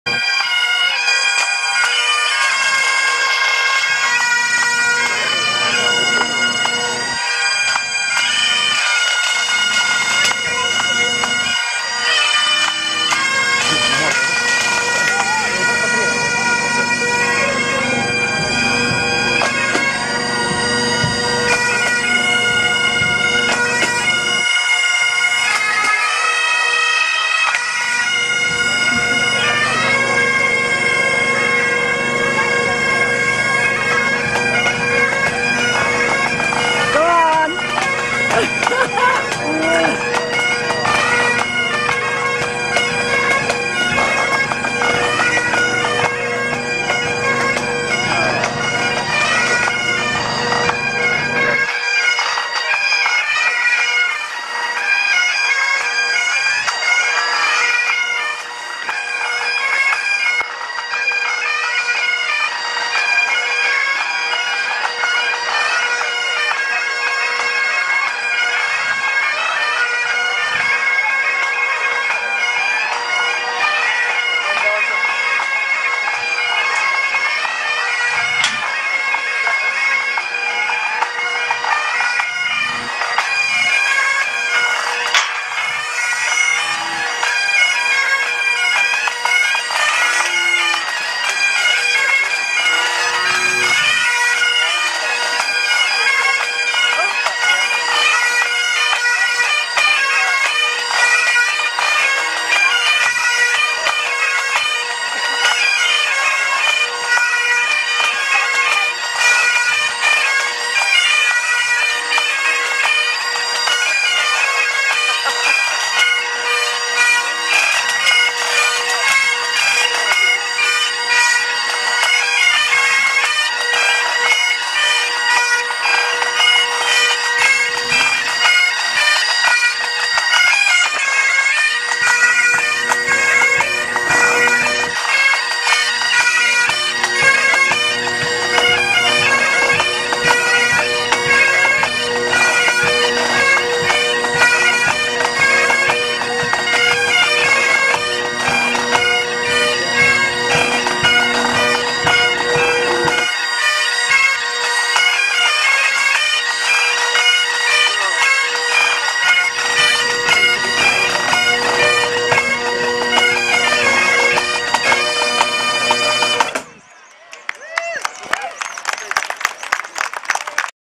South Uist Agricultural Show
Sgoil Lionacleit Pipe Band (Liniclate School) 3 of 4
How they managed to play in the heat I'll never know. A tiny breeze was floating by now and then, carrying the haunting music on its wings. Beautiful.